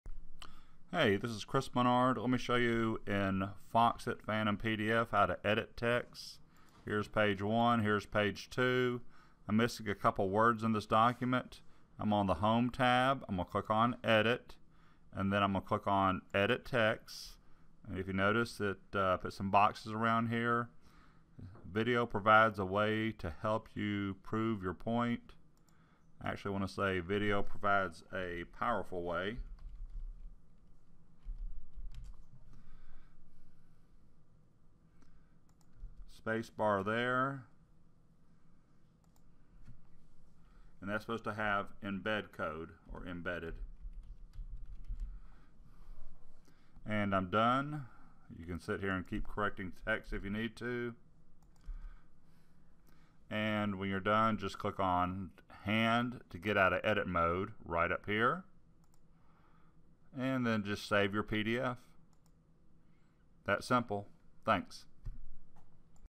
Edit Text with Foxit Phantom PDF by Chris Menard
Click on the page where you want to edit text. On the Ribbon, click the Edit tab at the top (two tabs down from the Home tab), and then click on Edit Text. After editing text, click on Hand in the top left and click Save.

-- EQUIPMENT USED
○ My camera